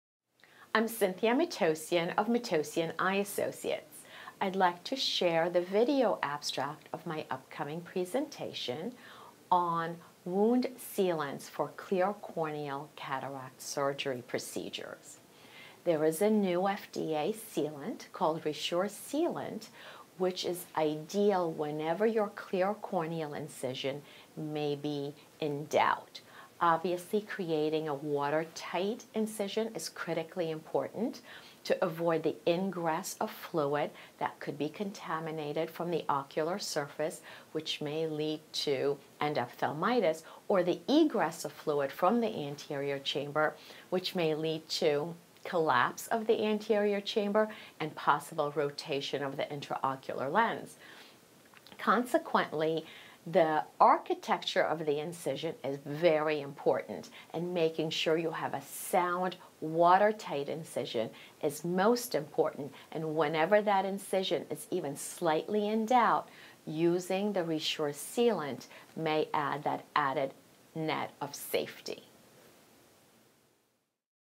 Cataract surgery and methods of wound closure – Video abstract [ID 83552]
Video abstract of a review paper “Cataract surgery and methods of wound closure: a review” published in the open access journal Clinical Ophthalmology by Matossian C, Makari S, Potvin R.

Abstract: Clear corneal incisions are routinely used in cataract surgery, but watertight wound closure may not always be achieved, which can increase the risk for anterior chamber fluid egress or ocular surface fluid ingress. A new US Food and Drug Administration-approved ocular sealant appears to have good efficacy in sealing clear corneal incisions; its use may be indicated when wound integrity is in question.